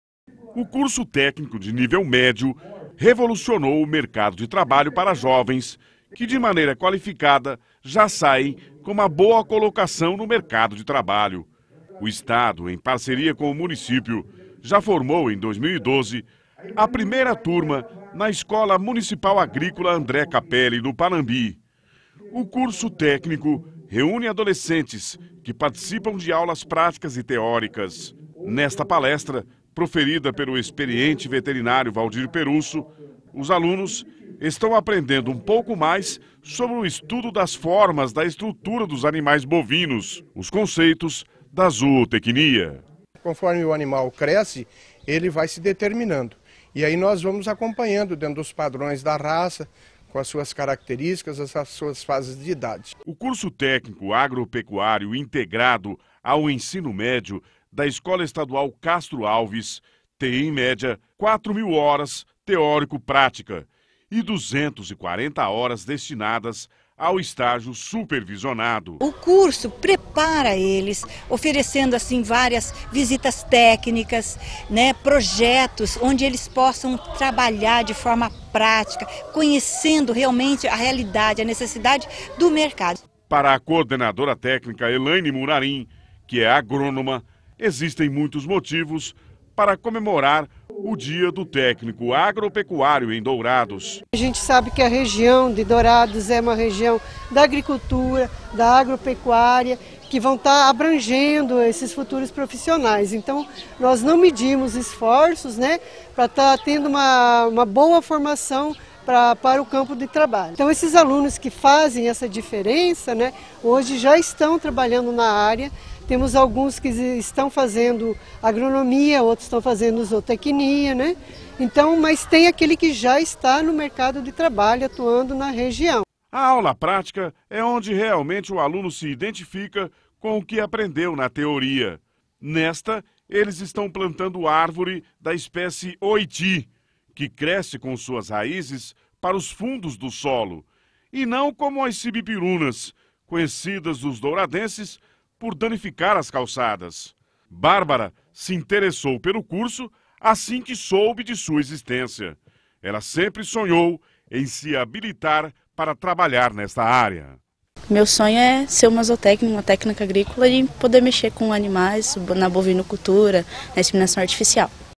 Alunos participam de palestras para comemorar o dia do técnico agropecuário
Em comemoração ao dia do técnico agropecuário, foi desenvolvida programação especial em escolas de Dourados que oferecem o curso de técnicos de nível médio. Durante o evento, os alunos assistiram palestras importantes e também participaram de aulas praticas.